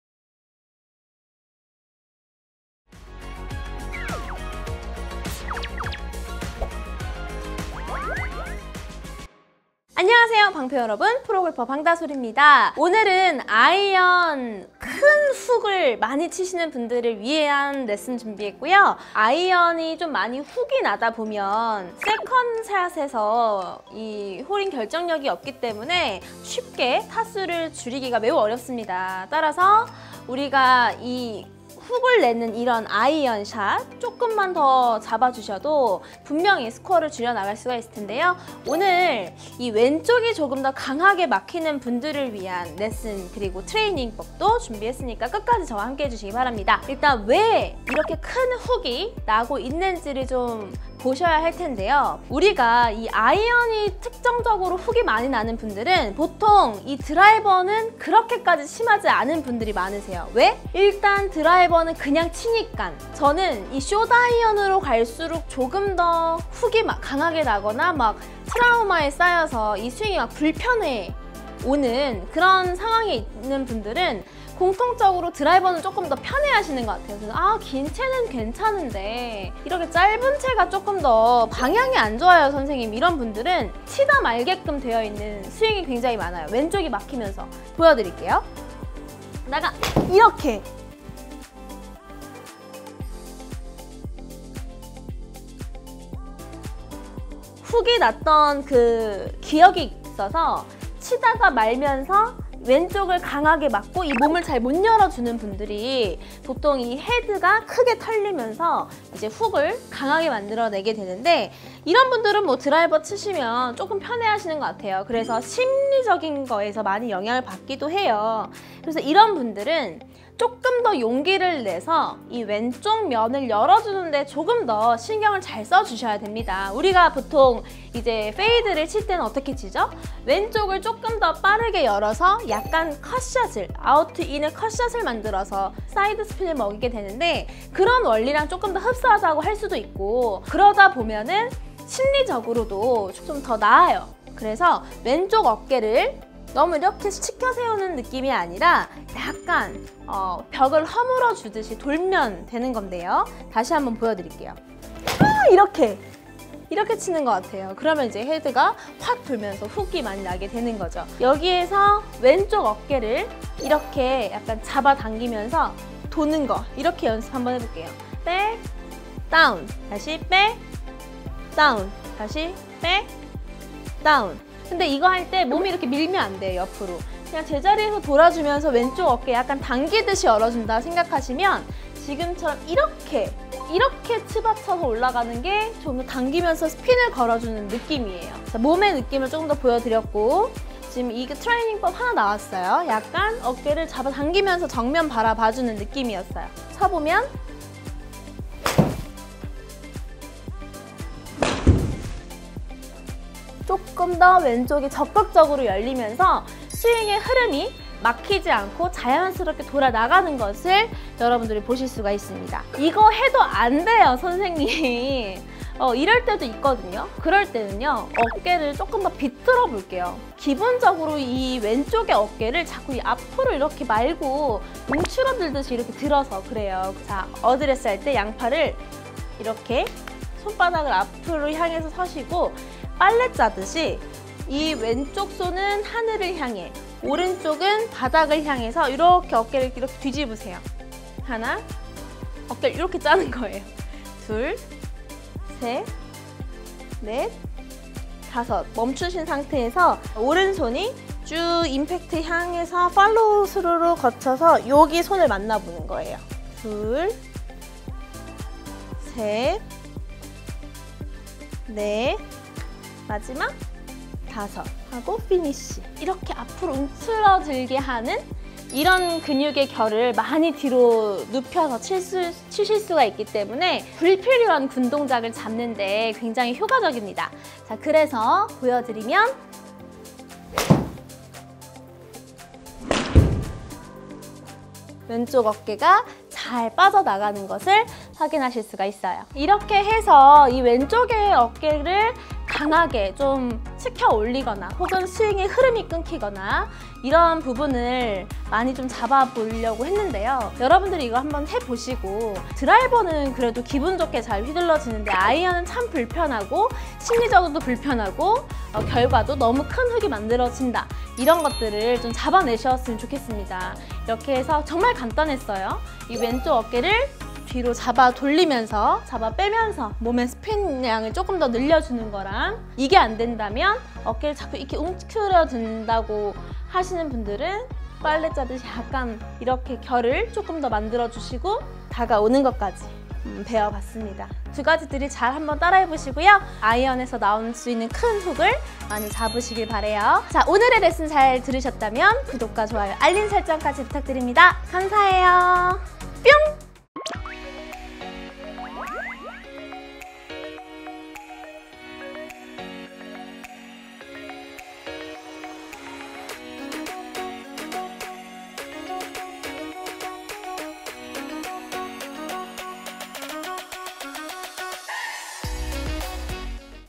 아이언 샷 | 큰 훅이 문제이신가요? [방프로 채널ㅣBDS클래스 EP.114]
아이언 샷 큰 훅을 치시는 분들을 위한 영상이에요!
물론, 큰 훅을 치시지 않은 분들도 이 영상을 보고 배우신다면
나중에 큰 도움이 될 거라고 생각됩니다. :)